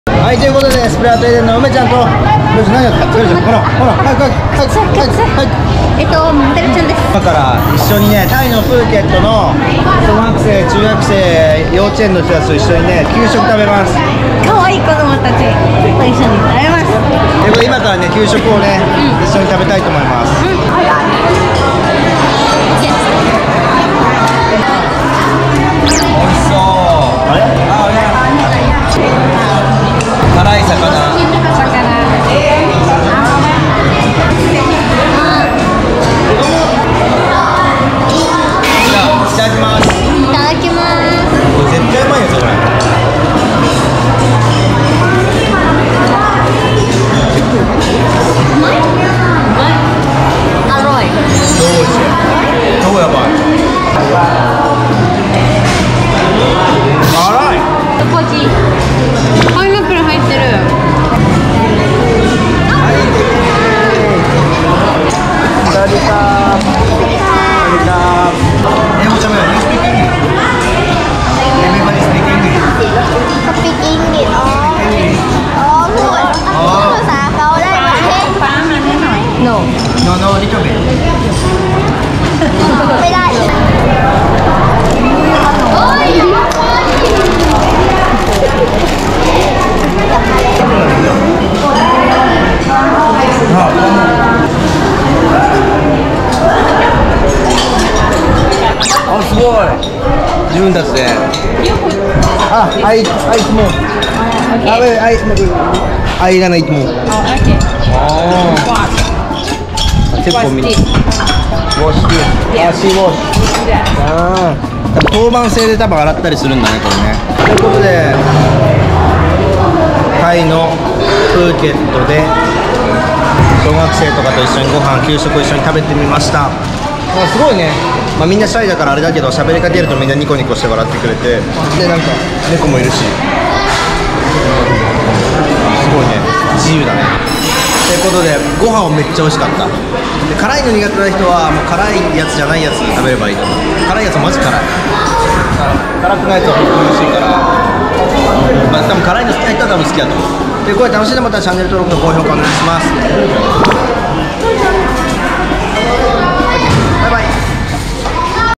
タイの小学校体験入学！子供たちと楽しい時間を過ごしました！
※この動画は去年撮影したものです